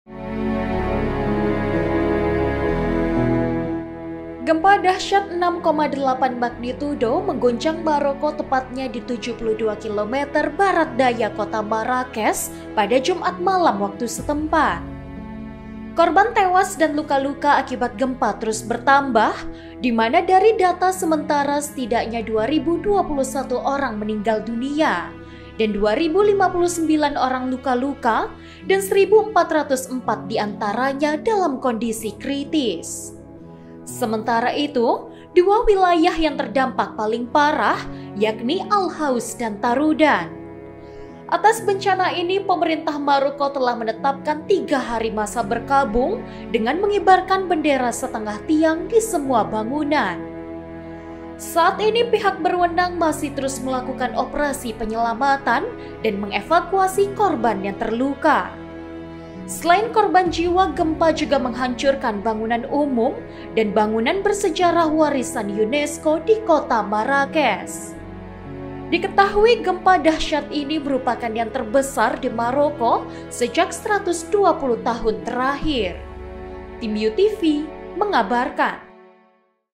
KORB4N T*WAS GEMPA MAROKO CAPAI 2.021 ORANG | U-NEWS
MAROKO - Gempa dahsyat Magnitudo 6,8 mengguncang Maroko pada Jumat malam waktu setempat. dari data semenetara, akibat bencana itu dilaporkan sebanyak 2.012 orang m*ninggal dunia.

Selain itu, sebanyak 2.059 orang luka-luka dan 1.404 diantaranya dalam kondisi kritis

Berikut Liputannya.